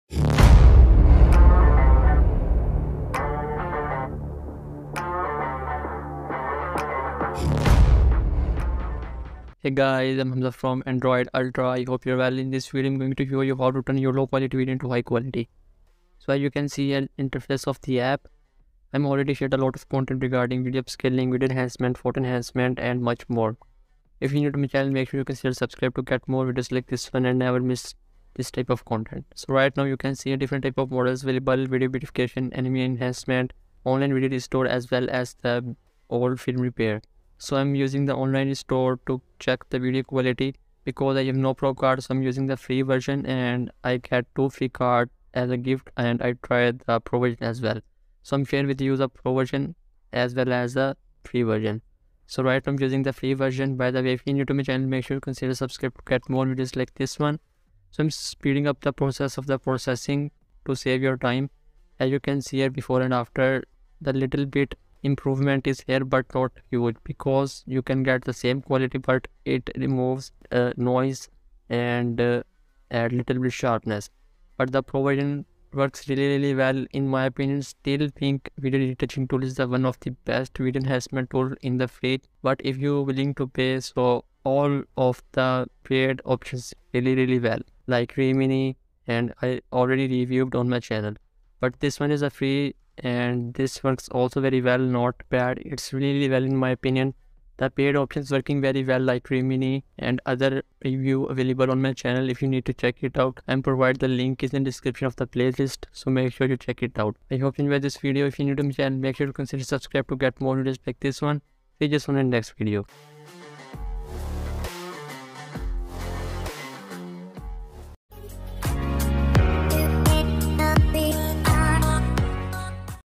Enhance Video Quality on Android - Video Enhancer App
Enhance Video Quality on Android. Video Enhancer App how to make low quality video to high quality on Mobile using app. It's really difficult to restore the video quality but now possible to Enhance video to High resolution upto 4K.